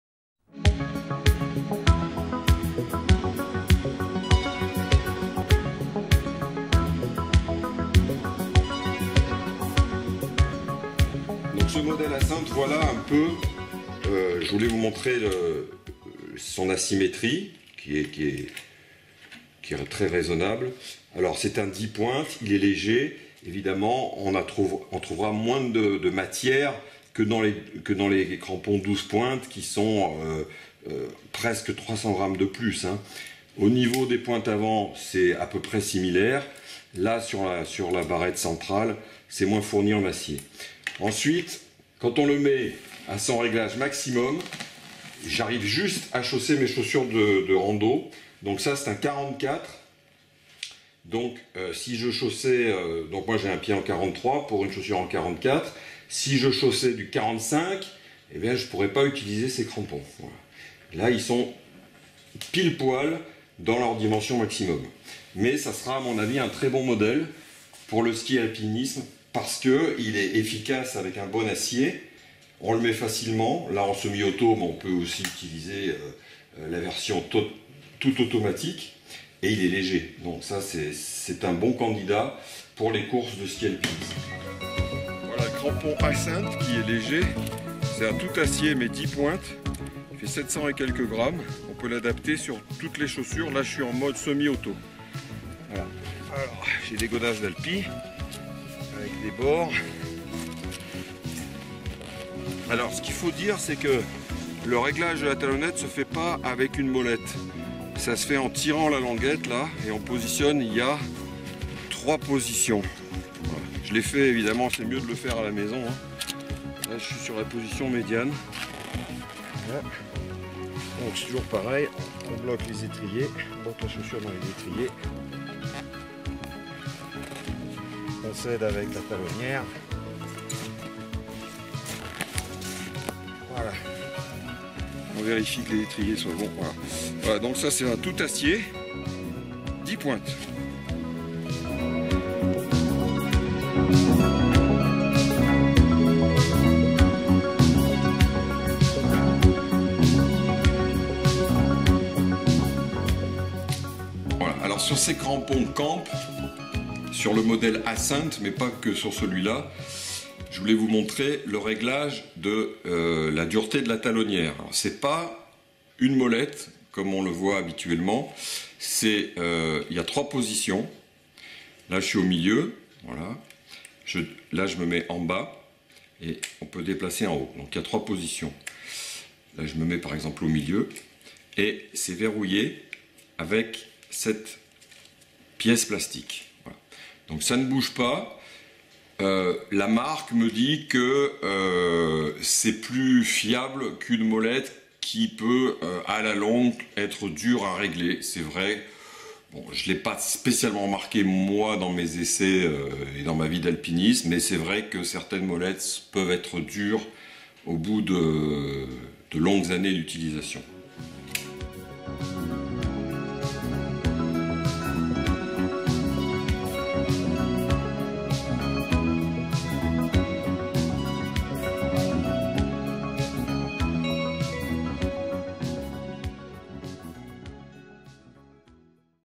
Crampon Ascent (Camp)
Présentation du crampon Ascent de Camp. Un 10 pointes tout acier assez léger et très polyvalent: utilisable en auto, semi-auto et aussi lanières (fixation disponibles séparément). Un bon modèle passe partout utilisable pour la randonnée et l'alpinisme modéré. Un peu limité pour le ski de rando par sa taille max. Le test complet